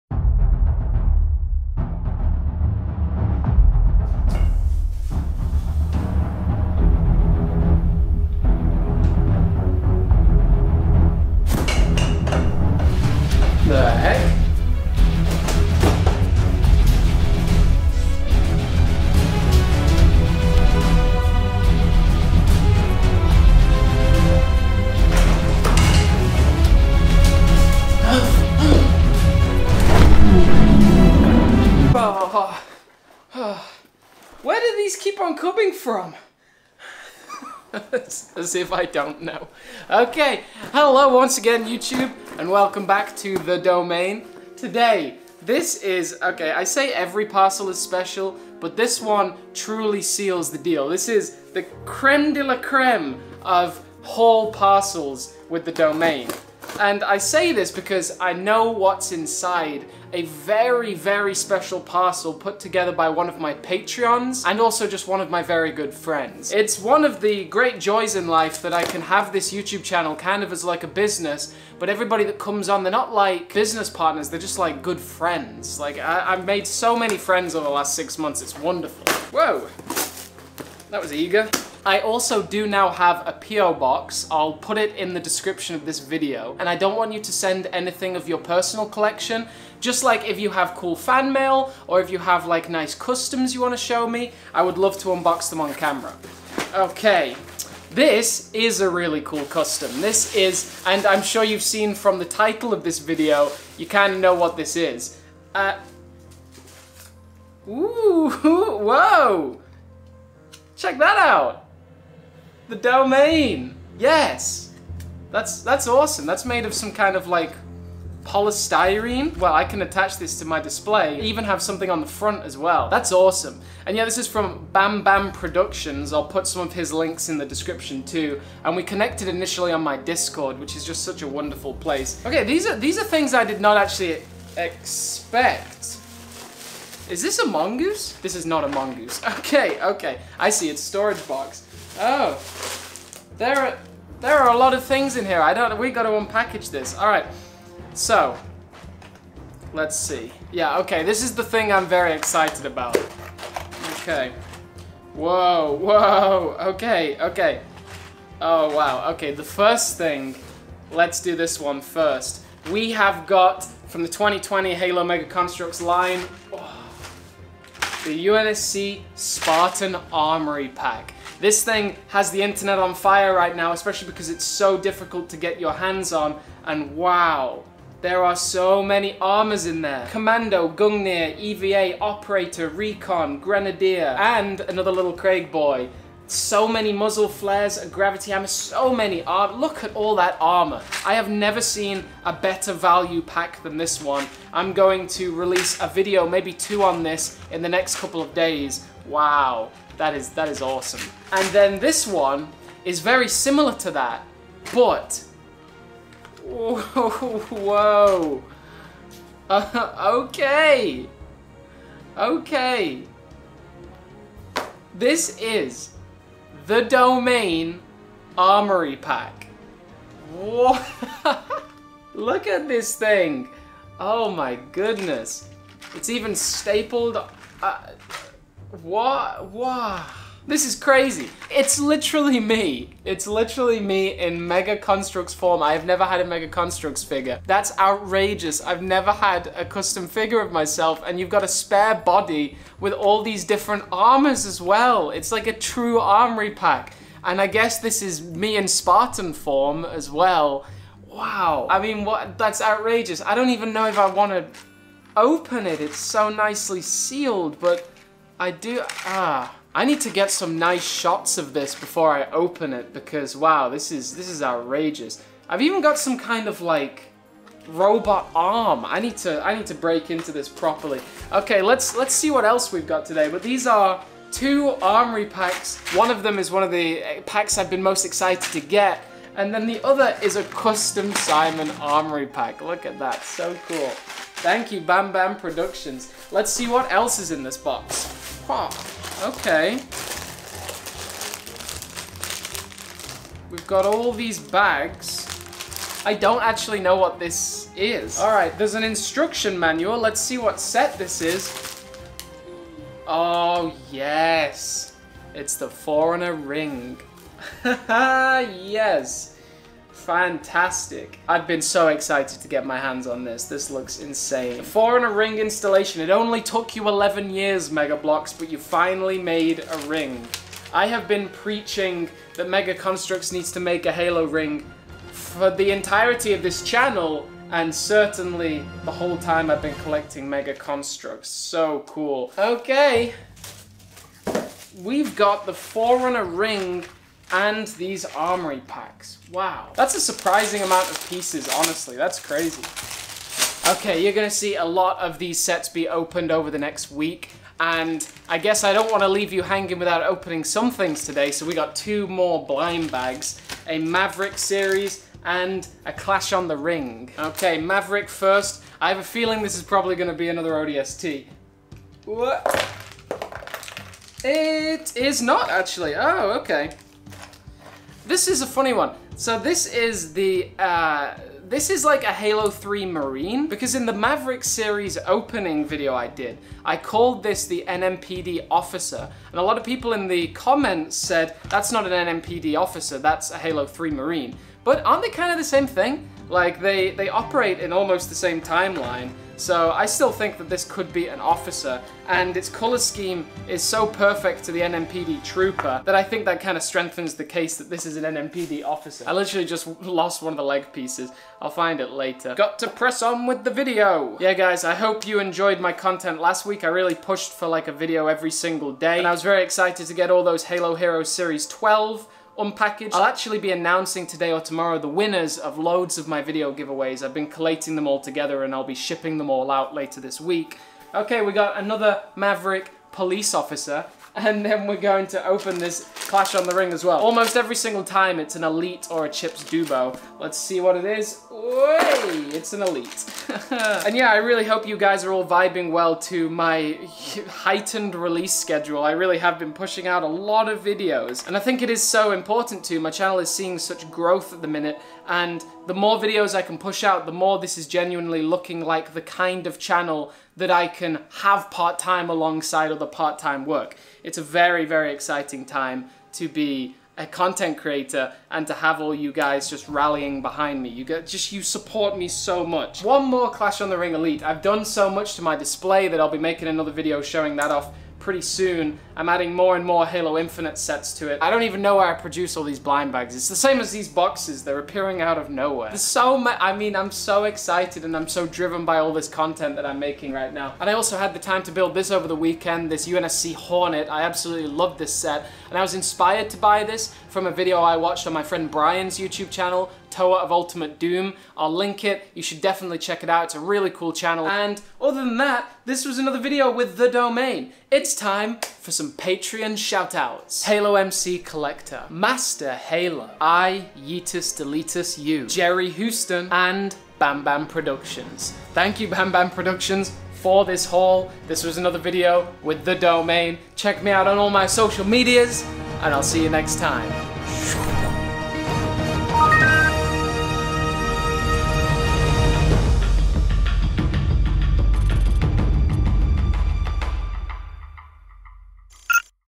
I CAN'T BELIEVE SOMEONE SENT ME THIS! Halo Mega Construx Haul
Wow, I honestly can't believe this was made for me. This community is AWESOME! Enjoy my latest Halo Mega Construx Haul!

Today's haul included some amazing new 202 sets, including the forerunner ring installation, UNSC Spartan Armoury Pack, blind bags, and a VERY secret set!

This channel is achieving greater and greater things and it is all thanks to all of my legendary subscribers.